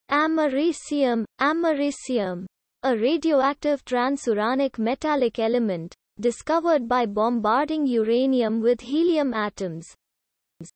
'Americium' Meaning and Pronunciation
Word: Americium
Meaning: a radioactive transuranic metallic element; discovered by bombarding uranium with helium atoms